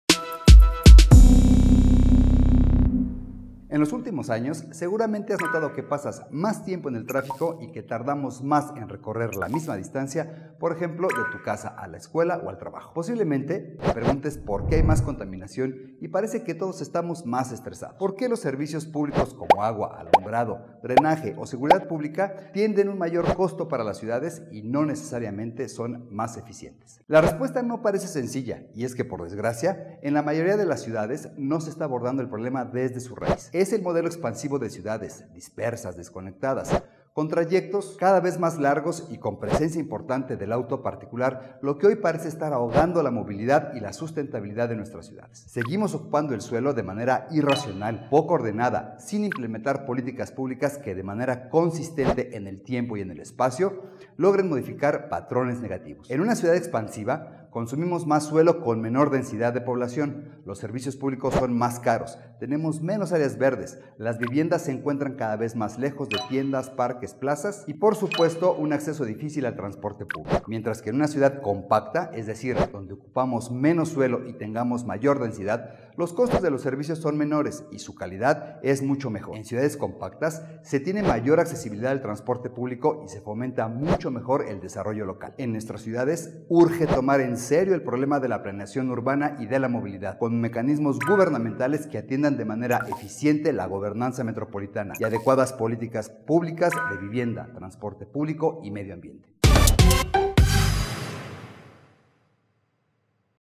Ciudad, movilidad ahogada - 88 segundos - Luis Olmos
¿Cuánto tiempo pasas metido en el tráfico?, ¿Sientes que cada vez tardas más en llegar a tu destino? No estás soñando, la ciudades cada vez se hacen más grandes y la forma de solucionar la movilidad les está quedando chica... ¿Hay solución? @aluisolmos te la da en 88segundos

¡Síguenos la pista!